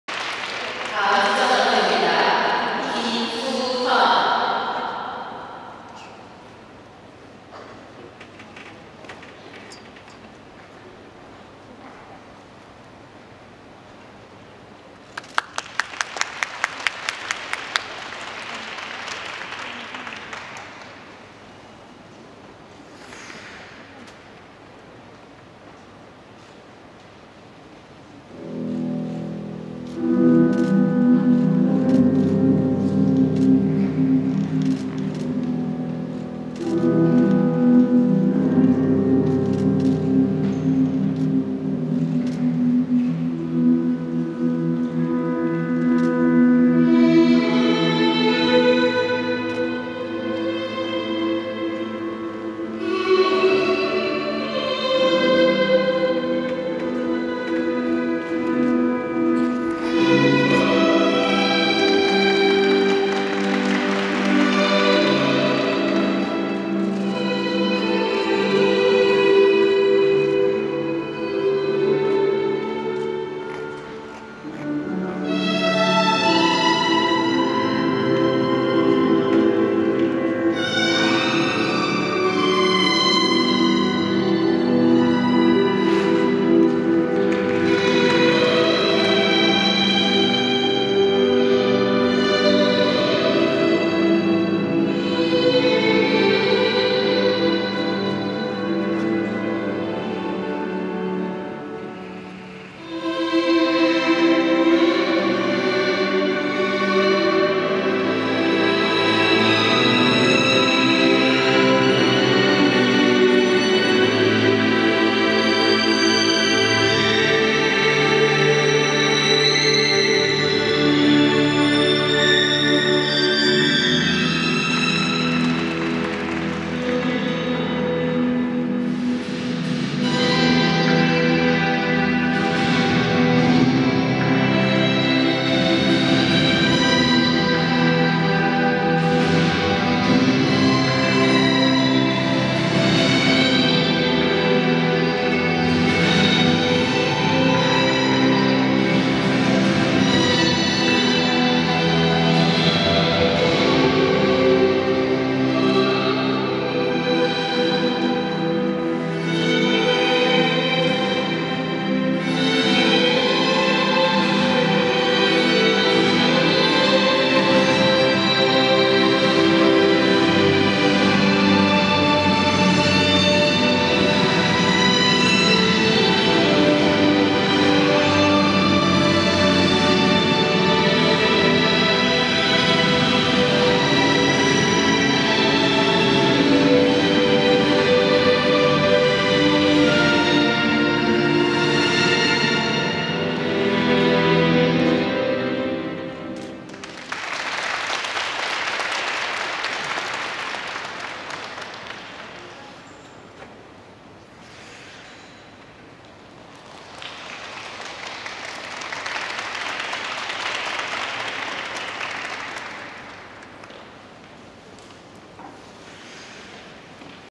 SP #10 김수현 SU HYUN KIM (00SEP) @ 2016년 피겨 랭킹대회 여자시니어
2016/10/15, 2016 KSU President Cup Ranking Competition Day2

2016/10/15, 목동 아이스링크, 2016년 전국남녀 피겨스케이팅 회장배 랭킹대회 2일차